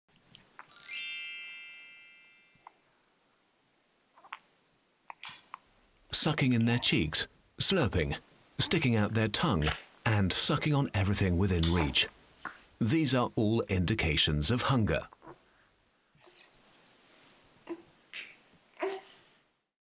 Sucking
The toddler sucks and slurps from hunger.